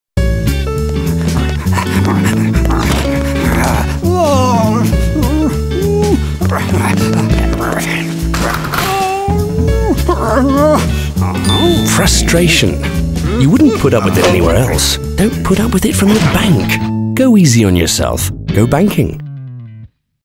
GO Banking - Toilet (Mixed by Paul Baxter)
This animation was produced for Y&R Gitam in South Africa.

Sound design and mix by Paul Baxter. Now working at Baxter Sound in Melbourne.